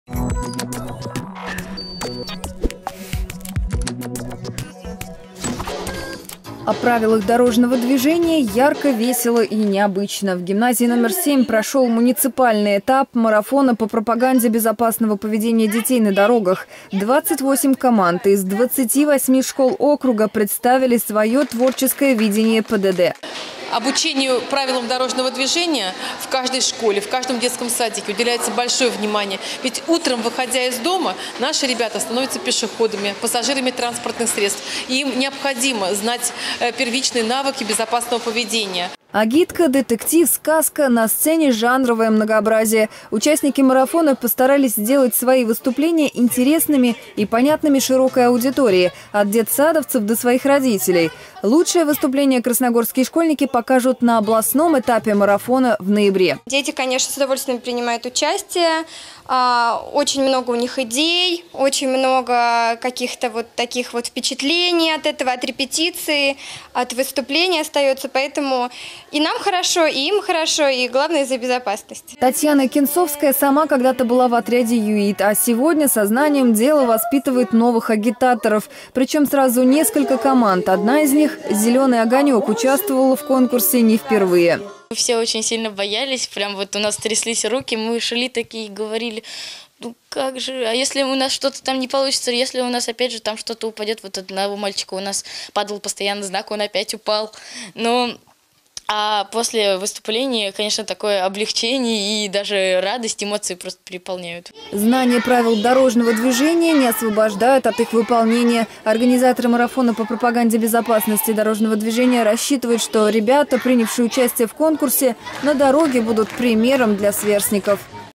КРТВ. Школьники о правилах дорожного движения
О правилах дорожного движения – ярко, весело и необычно. В гимназии №7 прошёл муниципальный этап марафона по пропаганде безопасного поведения детей на дорогах. 28 команд – из 28 школ округа представили своё творческое видение ПДД